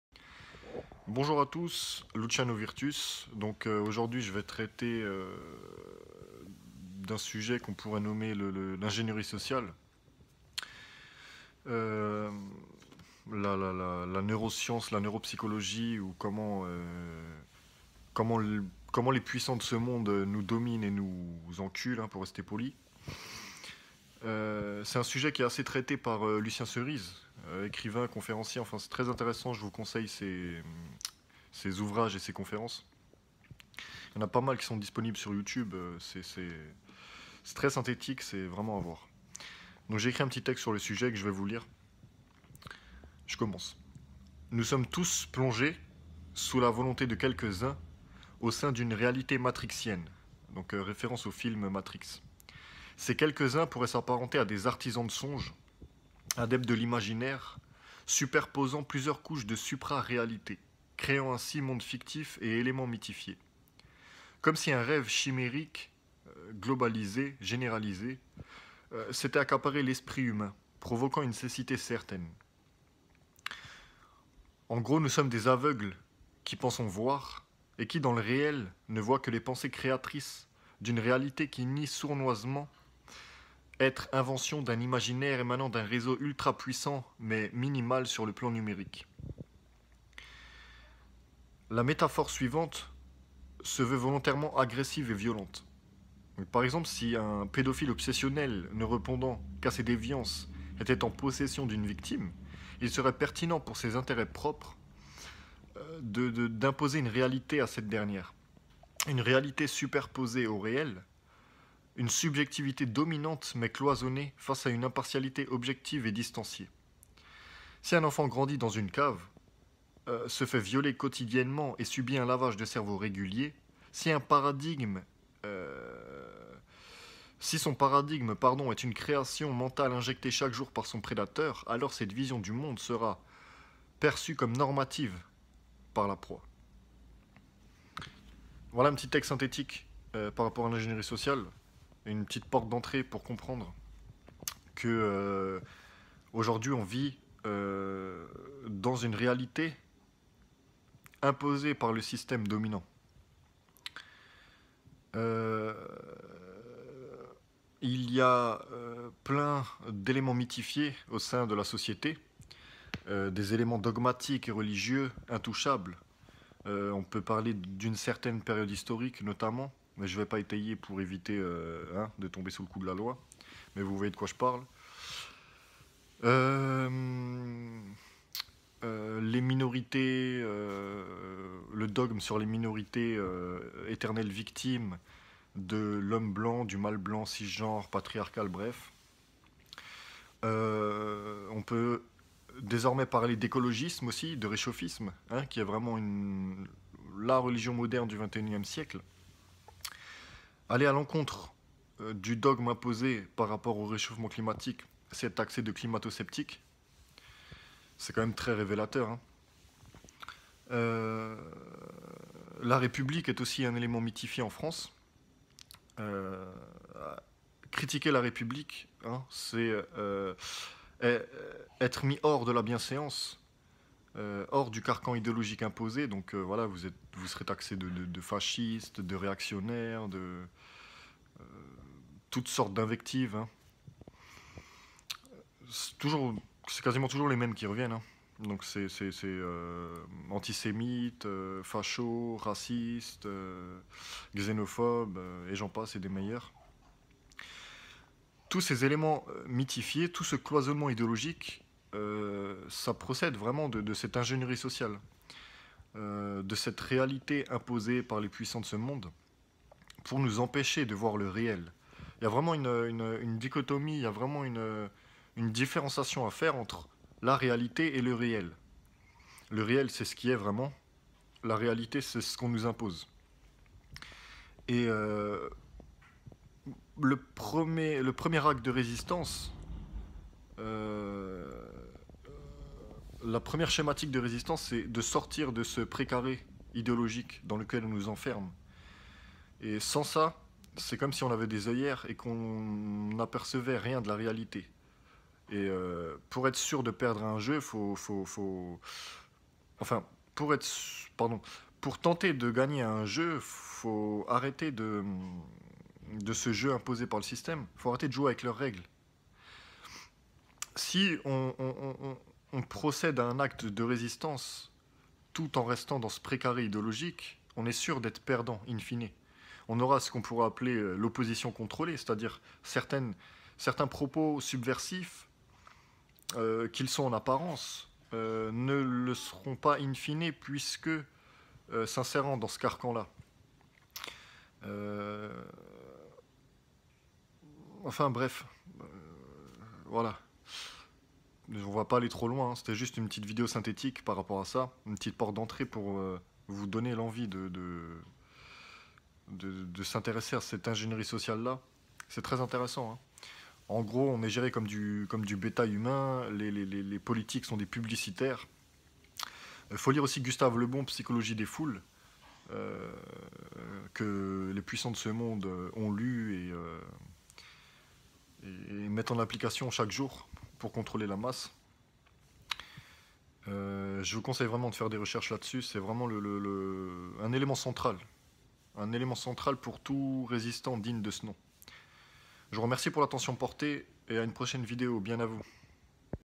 Quelques mots sur l’ingénierie sociale (la réalité face au réel)
Ou comment l’oligarchie ploutocrate s’infiltre dans nos cerveaux pour nous faire aller où ils veulent qu’on aillent.